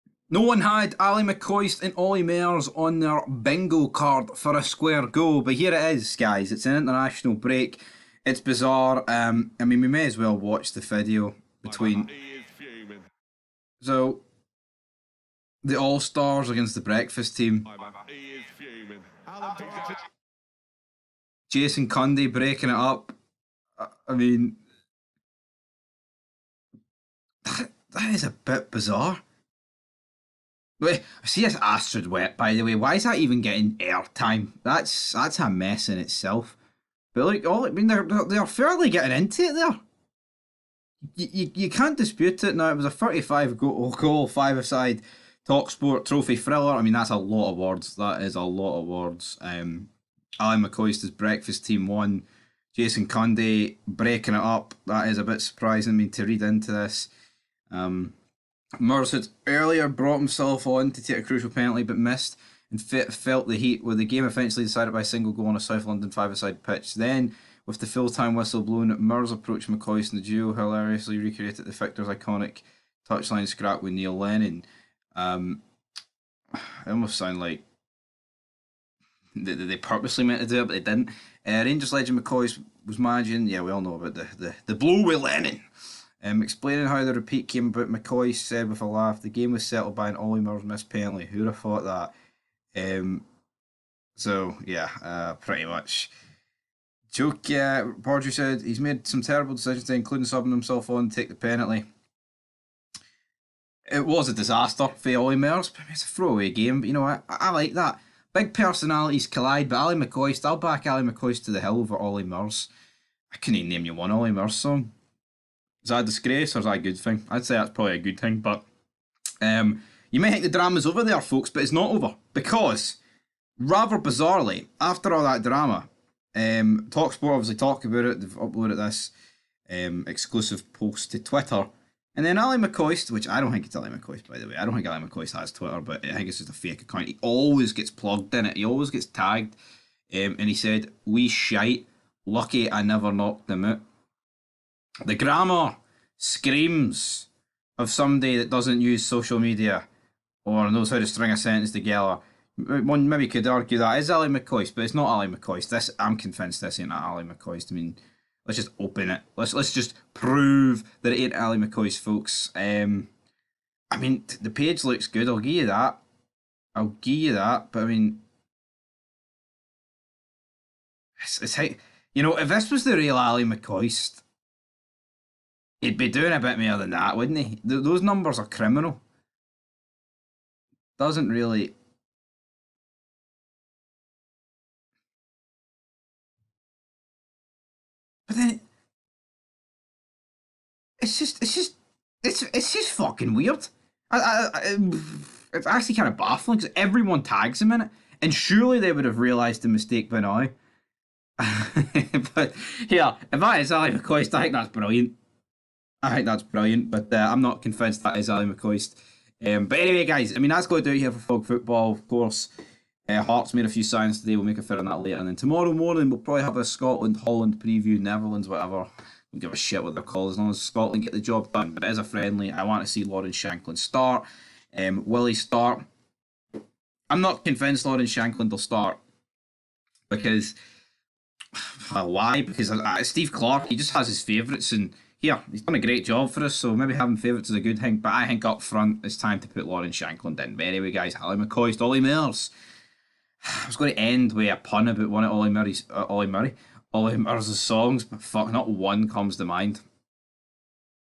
ALLY MCCOIST & OLLY MURS COME TO BLOWS... RECREATE LENNON & MCCOIST SPAT!
BIG CONFRONTATION!